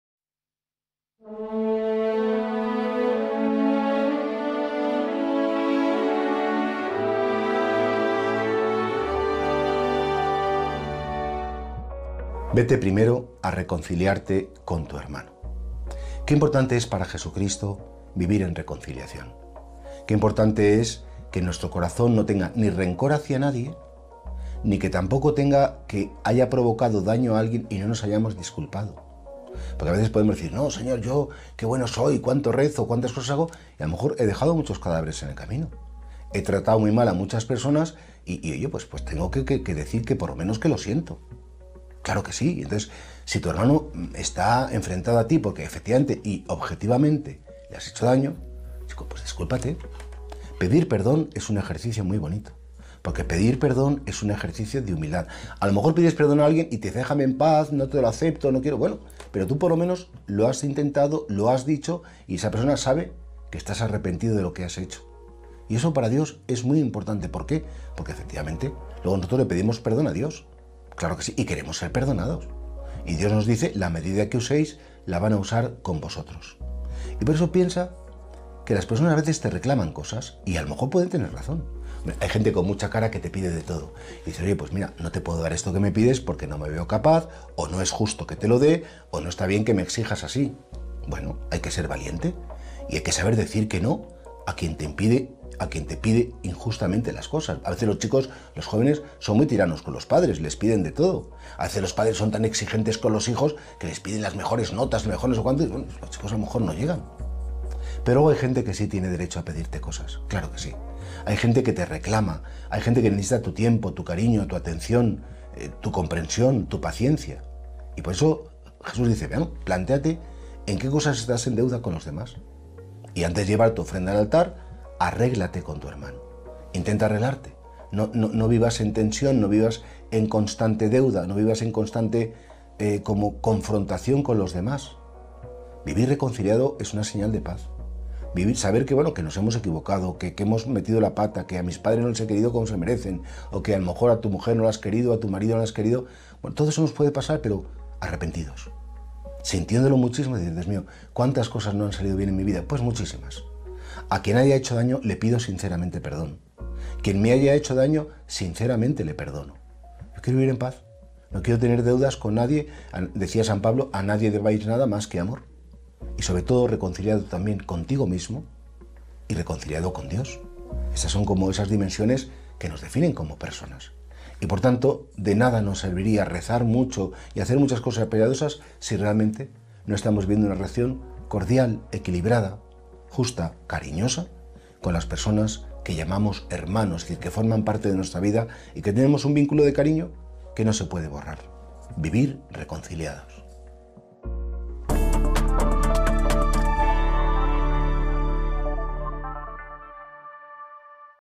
03.03.2023 - Palabra de Vida «Vete primero a reconciliarte con tu hermano» Por P Jesús Higueras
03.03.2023 - Palabra de Vida «Vete primero a reconciliarte con tu hermano»   Por P  Jesús Higueras